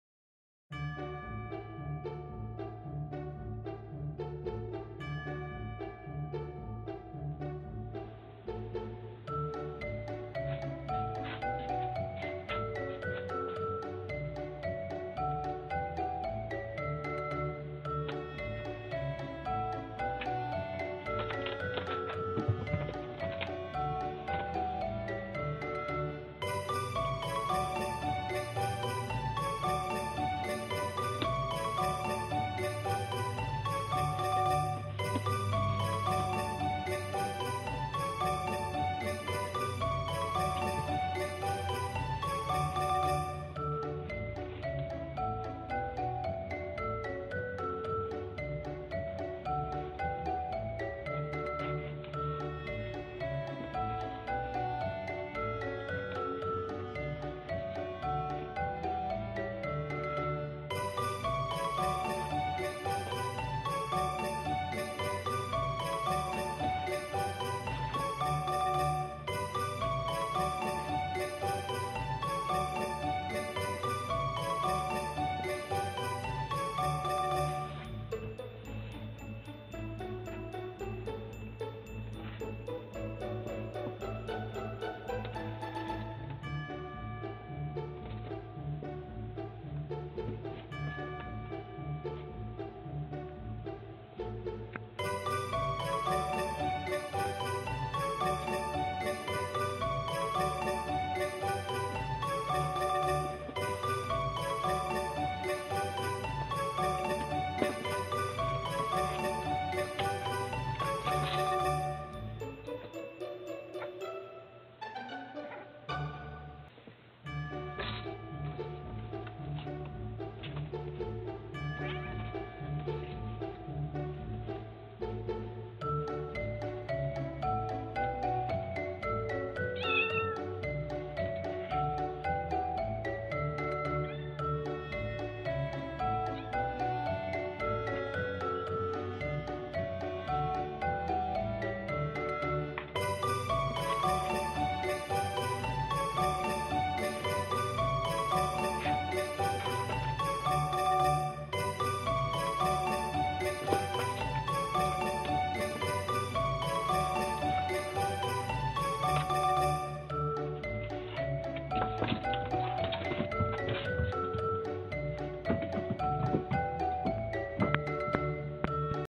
ビールの段ボール箱で遊ぶ子猫2匹Two kittens which are idle with a cardboard box of the beer
冷蔵庫の横に置いている第3のビール。まだ入ってるんですけれど猫の遊び場になってます。